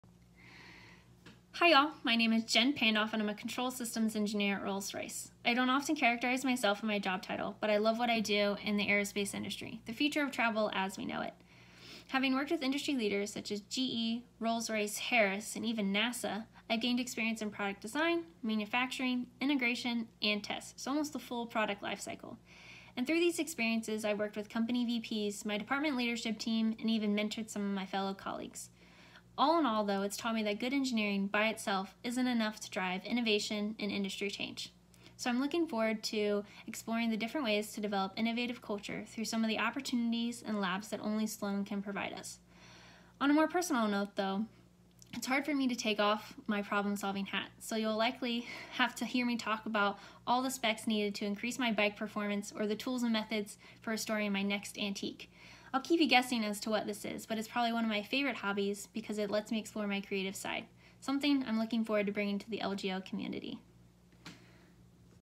MIT LGO Video Statement
A quick introduction and why I’m excited to apply to MIT’s LGO program as part of the Class of 2023!